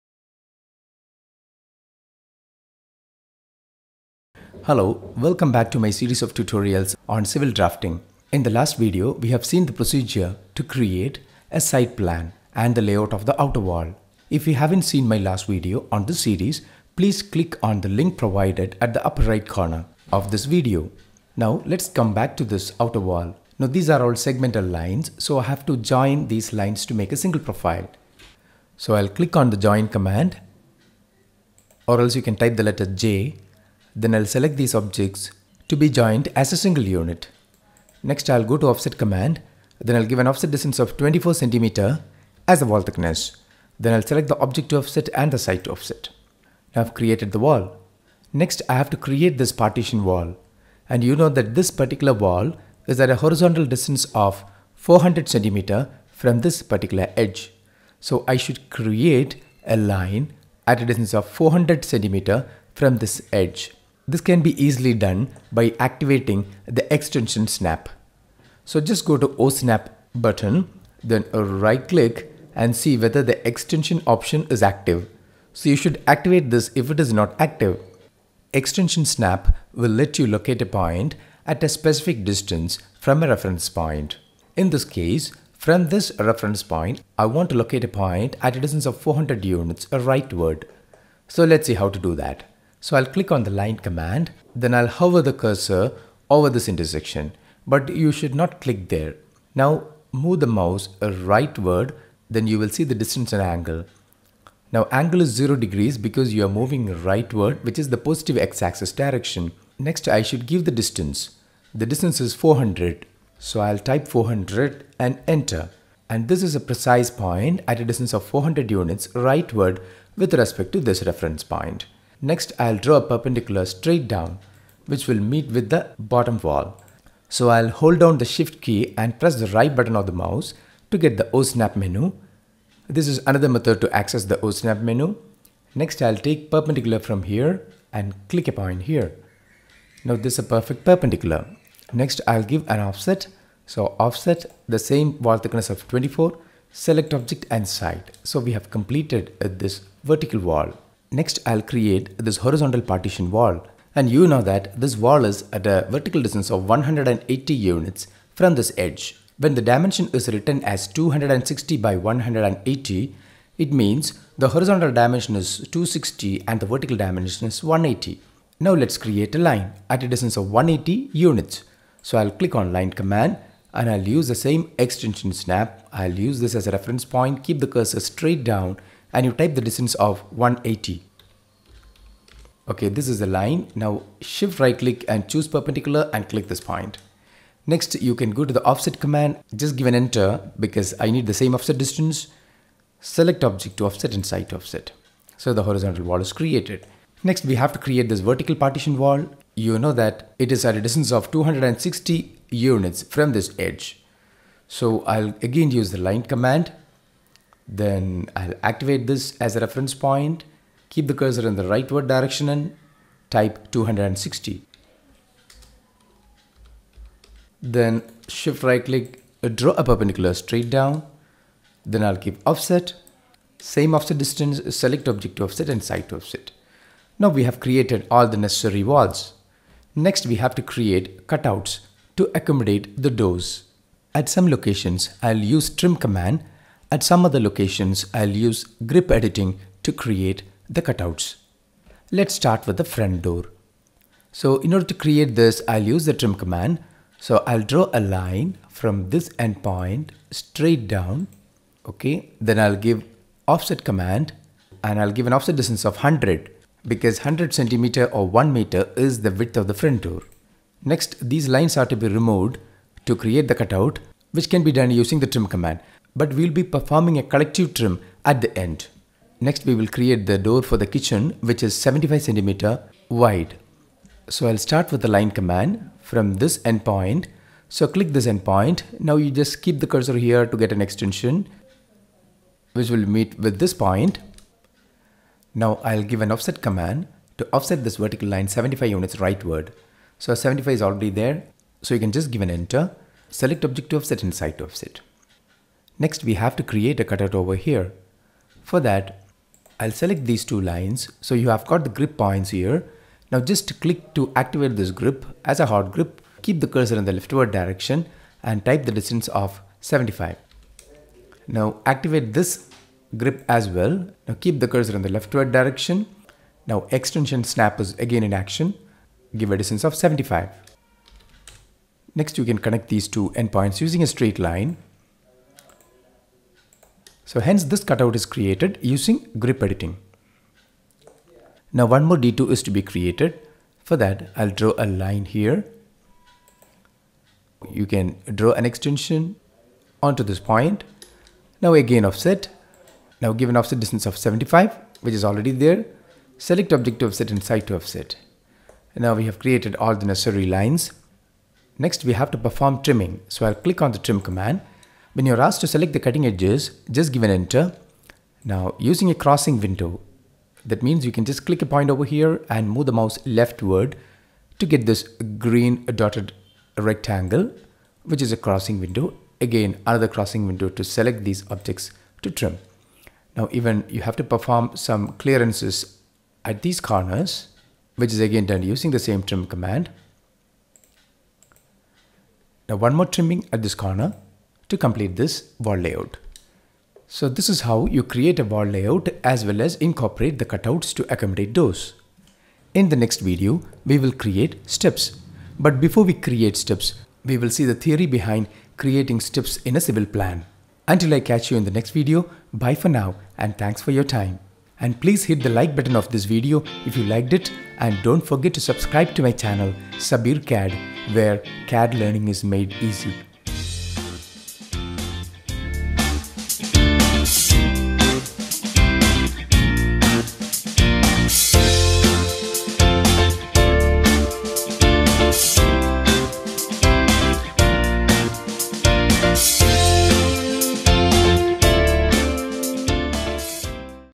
AutoCAD FLOOR PLAN | MAKE WALL LAYOUT USING AUTOCAD
AUTOCAD FLOOR  PLAN - This tutorial video teaches the best procedure to create wall layout of A Civil drawing using AutoCAD  .